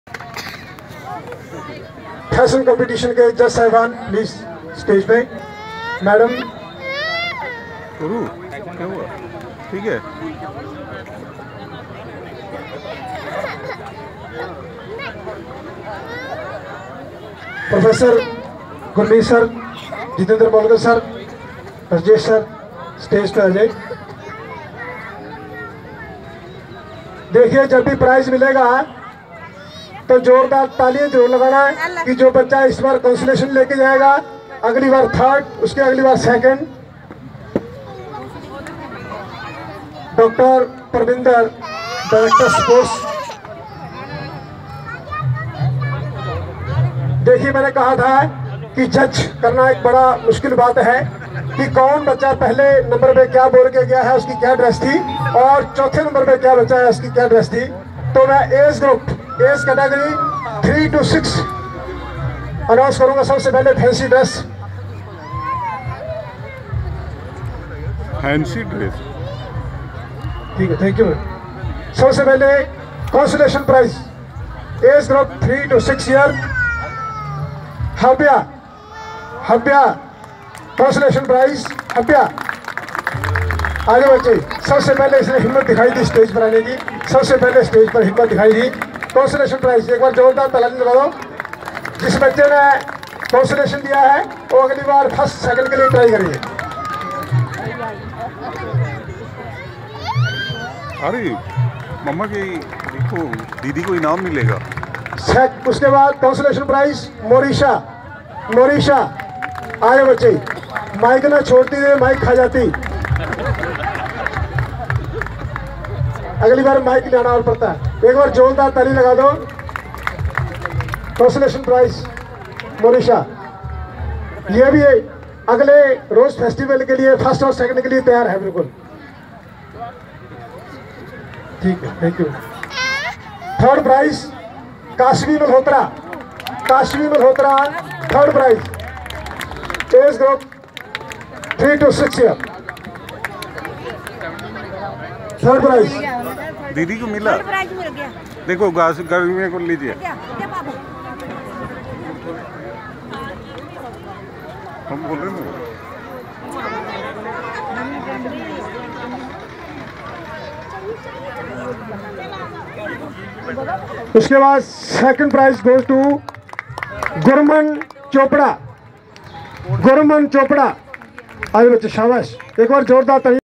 Rose Festival Punjab University
Kashvi Malhotra daughter of Dr  Anshoo Malhotea got third prize in rose festival. Indeed matter of pride.